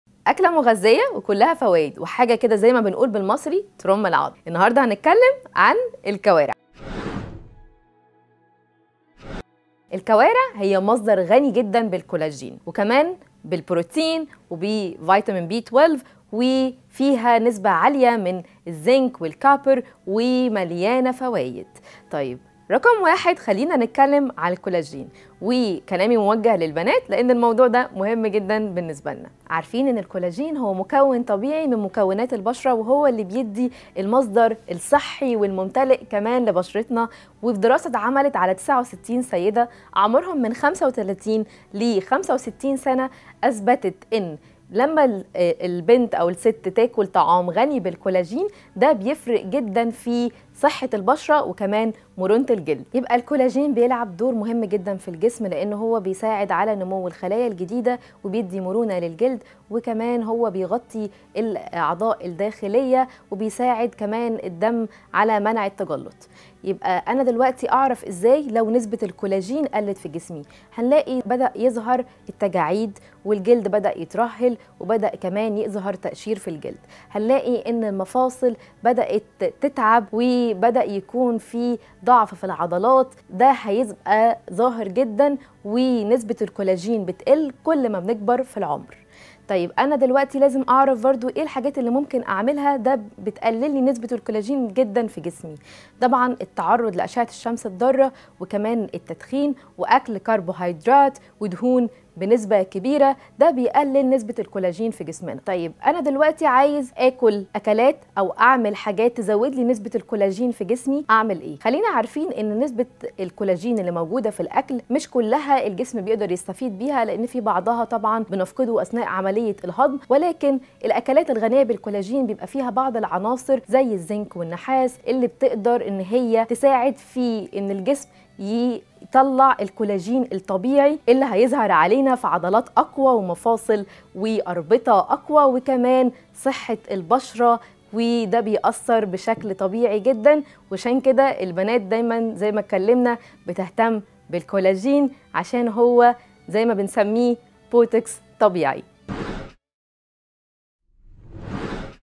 دكتورة أكل - فوائد مذهلة للكوارع
فوائد مذهلة للكوارع

مع الدكتورة مي تاج الدين استشاري سلامة الغذاء